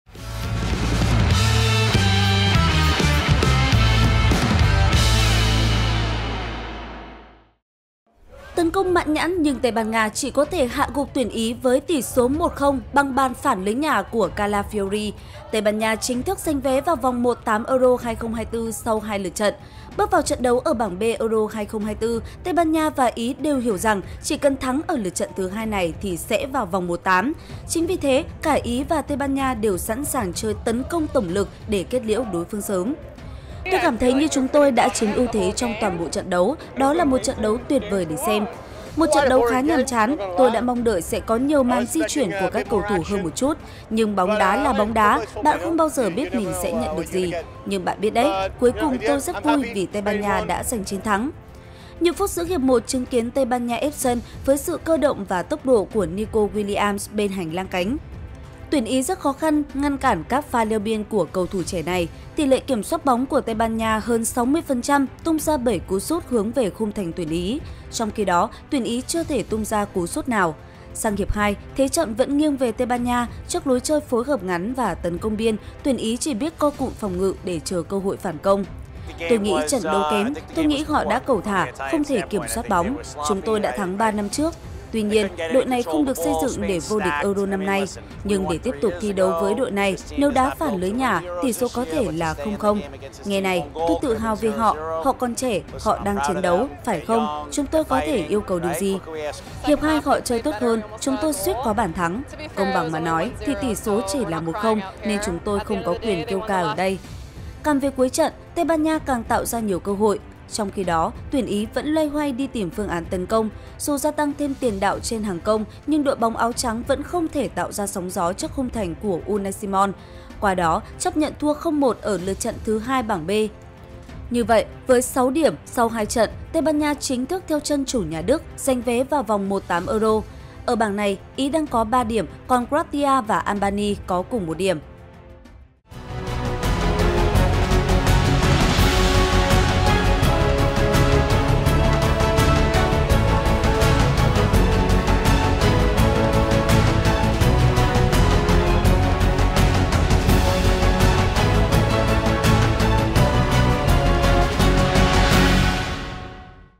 Kết quả Euro 2024: Đội bóng “thiên thanh” đau đớn nhường vé vào vòng 1/8 sớm cho Tây Ban Nha
PLO | Kết quả Euro 2024: Đội bóng “thiên thanh” đau đớn nhường vé vào vòng 1/8 sớm cho Tây Ban Nha
Tấn công mãn nhãn nhưng Tây Ban Nha chỉ có thể hạ gục tuyển Ý với tỷ số 1-0 bằng bàn phản lưới nhà của Calafiori, Tây Ban Nha chính thức giành vé vào vòng 1/8 Euro 2024 sau 2 lượt trận. Bước vào trận đấu ở bảng B Euro 2024, Tây Ban Nha và Ý đều hiểu rằng chỉ cần thắng ở lượt trận thứ 2 này thì sẽ vào vòng 1/8. Chính vì thế cả Ý và Tây Ban Nha đều sẵn sàng chơi tấn công tổng lực để kết liễu đối phương sớm.
Những phút giữa hiệp 1 chứng kiến Tây Ban Nha ép sân, với sự cơ động và tốc độ của Nico Williams bên hành lang cánh, tuyển Ý rất khó ngăn cản các pha leo biên của cầu thủ trẻ này. Tỷ lệ kiểm soát bóng của Tây Ban Nha hơn 60% tung ra 7 cú sút hướng về khung thành tuyển Ý, trong khi đó tuyển Ý chưa thể tung ra cú sút nào. Sang hiệp 2, thế trận vẫn nghiêng về Tây Ban Nha, trước lối chơi phối hợp ngắn và tấn công biên, tuyển Ý chỉ biết co cụm phòng ngự để chờ cơ hội phản công. Càng về cuối trận, Tây Ban Nha càng tạo ra nhiều cơ hội, trong khi đó tuyển Ý vẫn loay hoay đi tìm phương án tấn công, dù gia tăng thêm tiền đạo trên hàng công nhưng đội bóng áo trắng vẫn không thể tạo ra sóng gió trước khung thành của Unai Simon, qua đó chấp nhận thua 0-1 ở lượt trận thứ 2 bảng B. Như vậy, với 6 điểm sau hai trận Tây Ban Nha chính thức theo chân chủ nhà Đức giành vé vòng vòng 1/8 Euro. Ở bảng này, Ý đang có 3 điểm, còn Croatia và Albania có cùng 1 điểm.